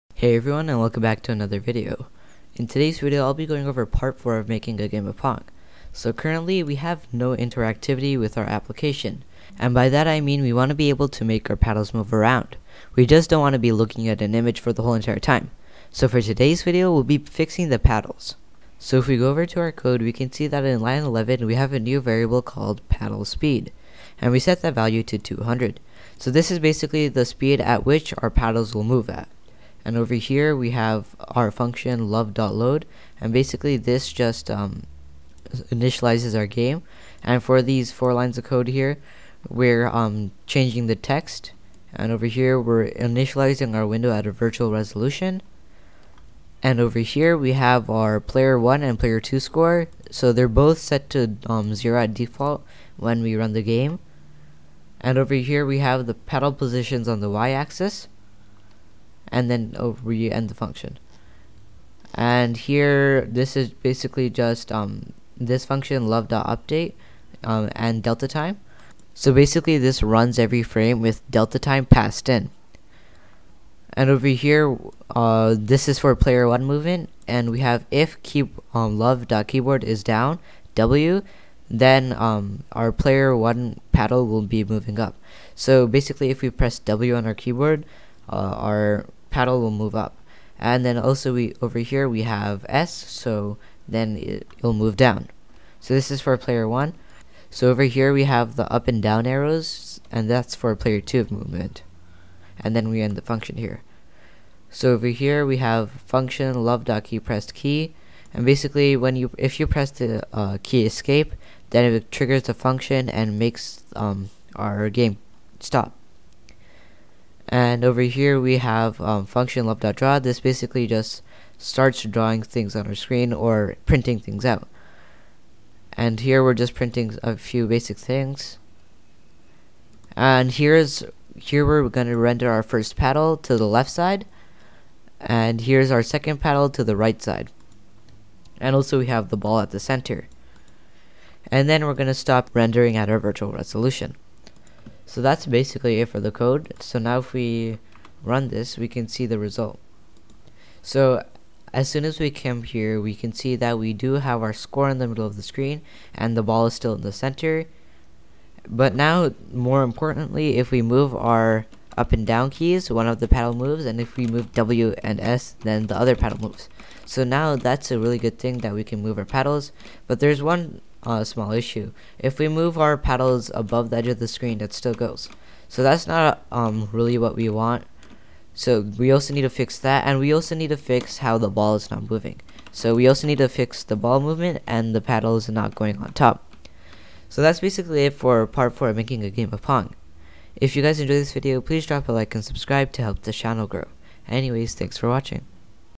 How To Create a Game of Pong With Programming (Lua & Love2d)
In this video, I showed how to create a game of pong with programming. The languages I used were Lua and Love2D. Note that this video is part 4 of creating a game of pong. Other videos will be coming out on this channel to complete the pong game.

How To Create a Game of Pong With Programming, How To Create a Game of Pong With Lua & Love2d, How To Create a Game of Pong Programming

I have a playlist with all the videos that I have made for creating the pong game